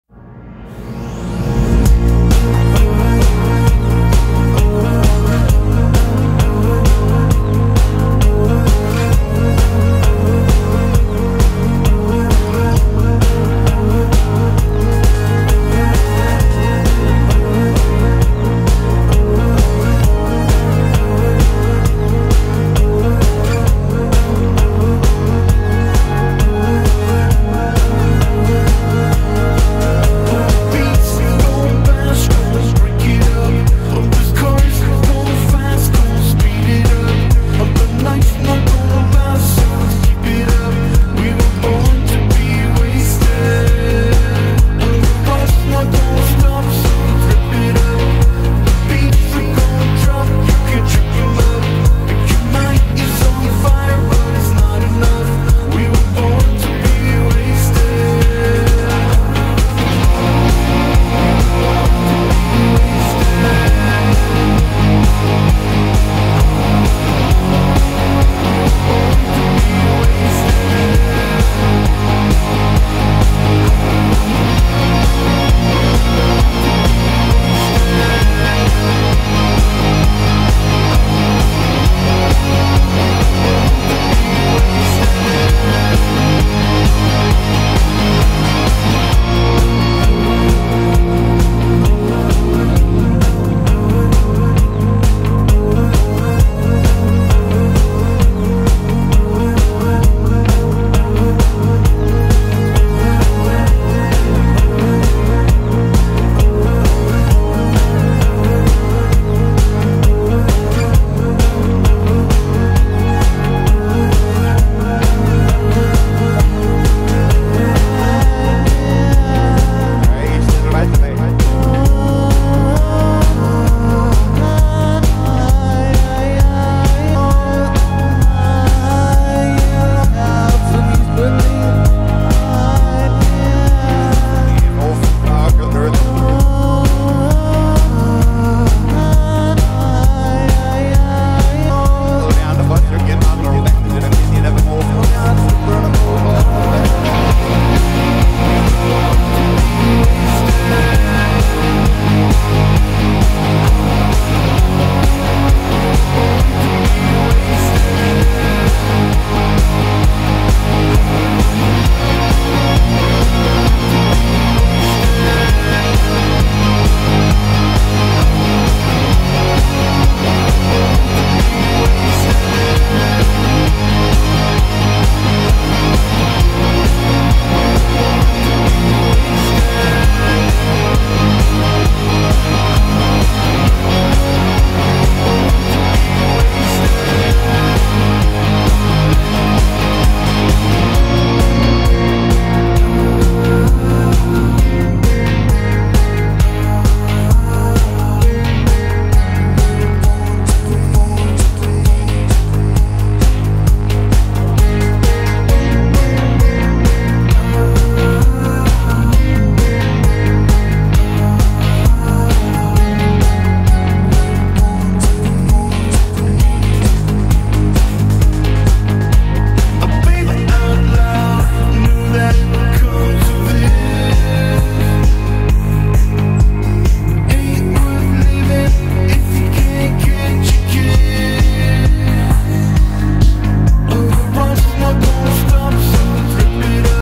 SDAS One Tournament.MOV
SDAS One setup with BCD/Tank Running Boat in 3-5 ft Seas.